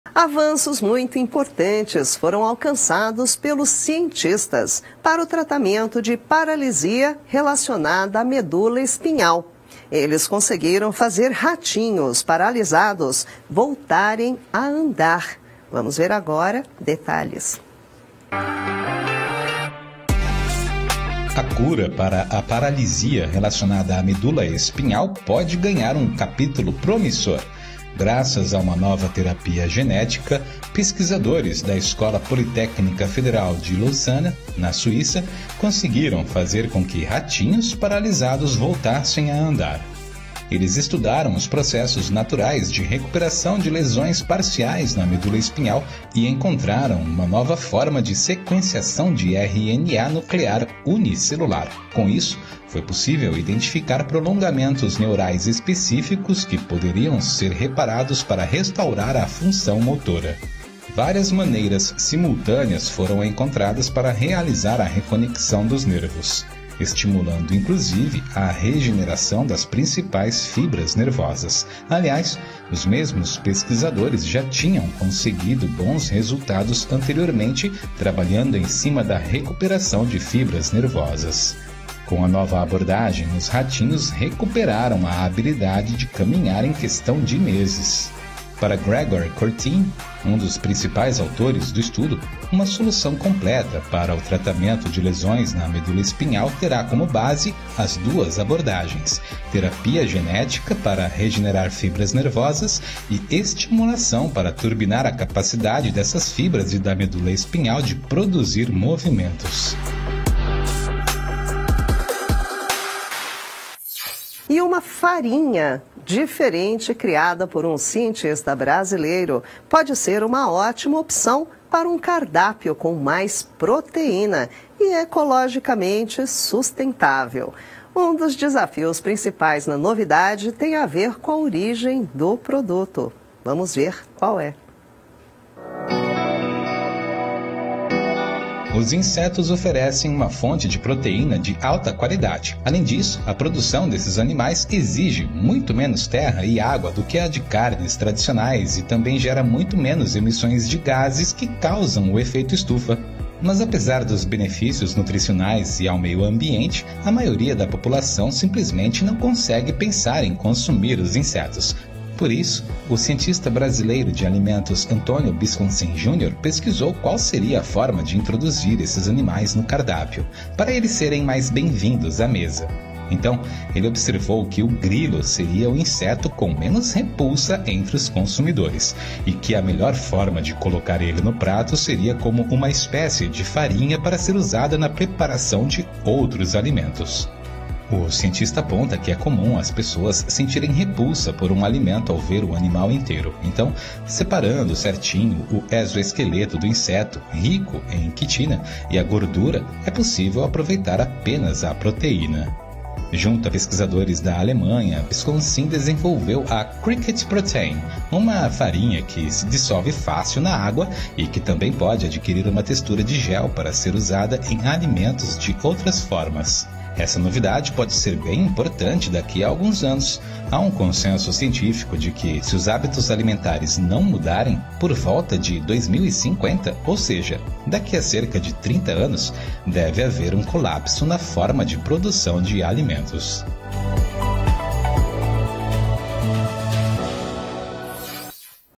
Nova terapia genética faz ratinhos paralisados voltarem a andar
Pesquisadores da EPFL desenvolvem técnica de terapia genética que permitiu que ratos com paralisia voltassem a andar. Descubra como funciona! 🧬🐭